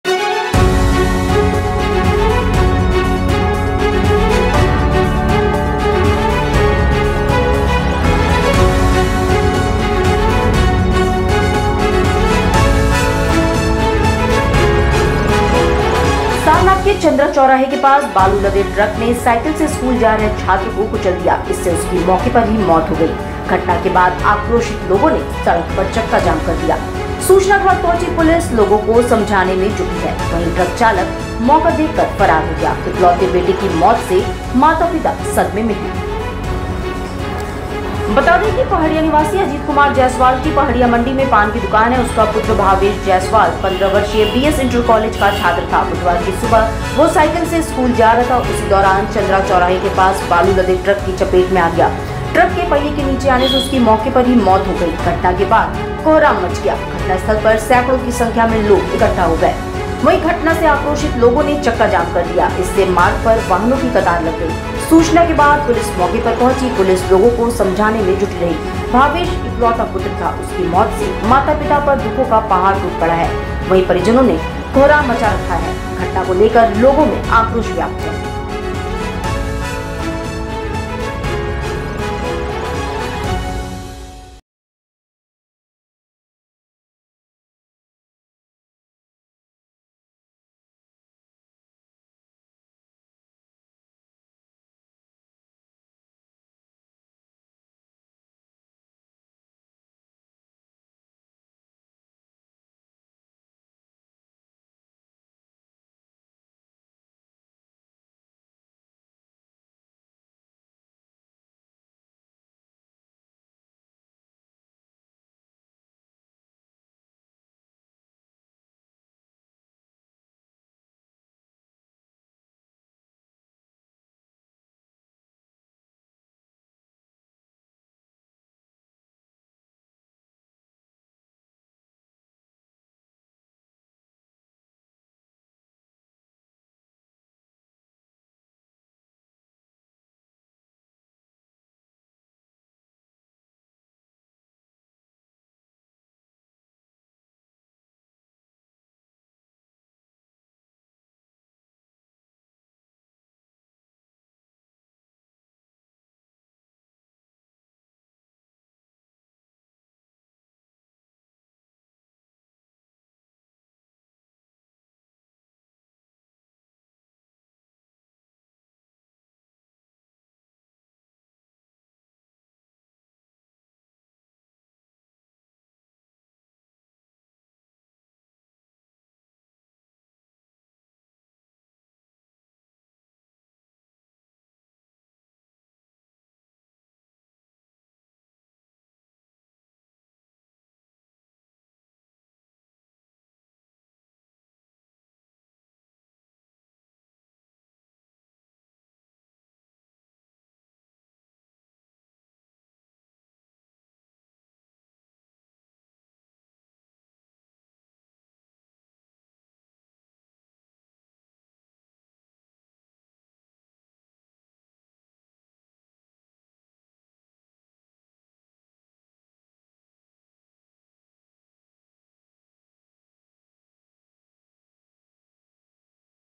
स्कूल जा रहे छात्र को ट्रक ने कुचला, मौत, लोगों ने किया चक्काजाम
Varanasi Education :
Banaras Hindu University :